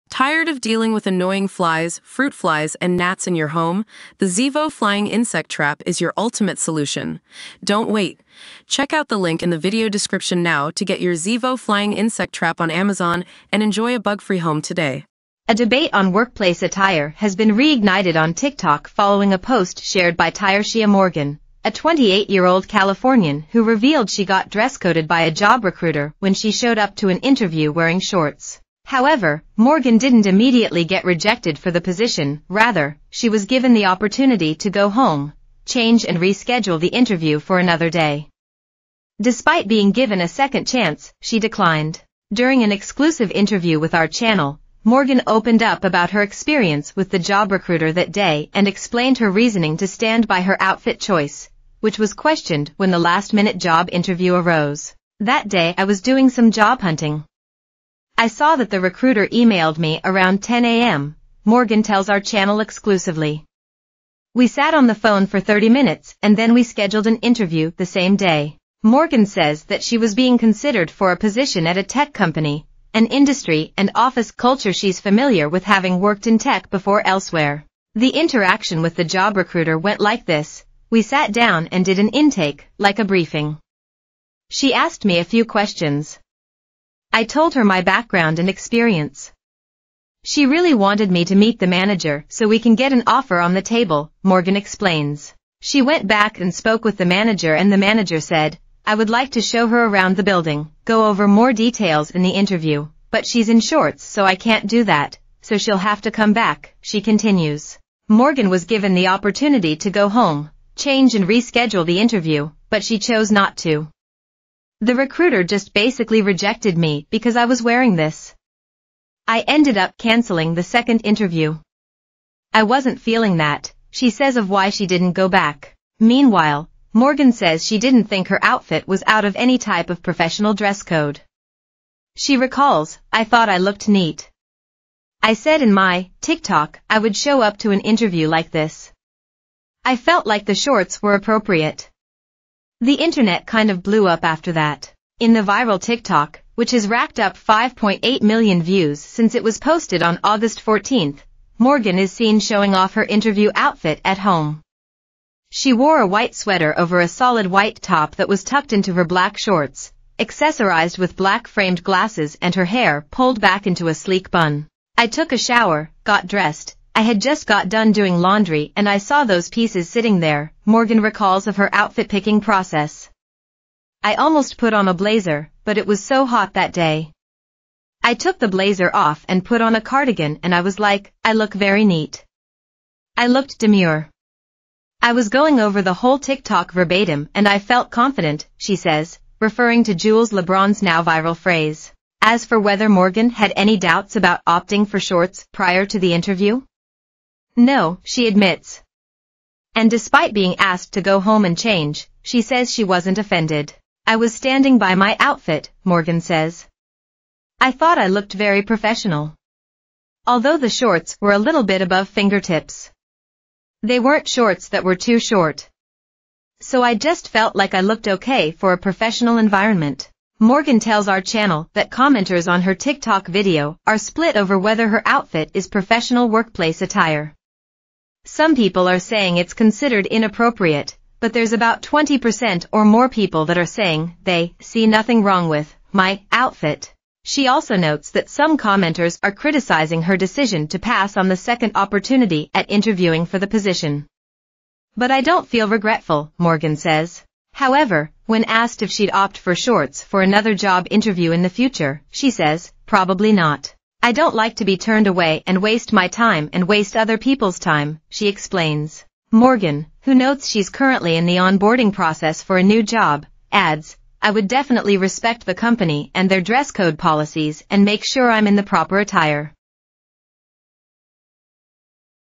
Woman shows up for job interview in shorts and is asked by recruiter to go home and change
Woman shows up for job interview in shorts and is asked by recruiter to go home and change (Exclusive)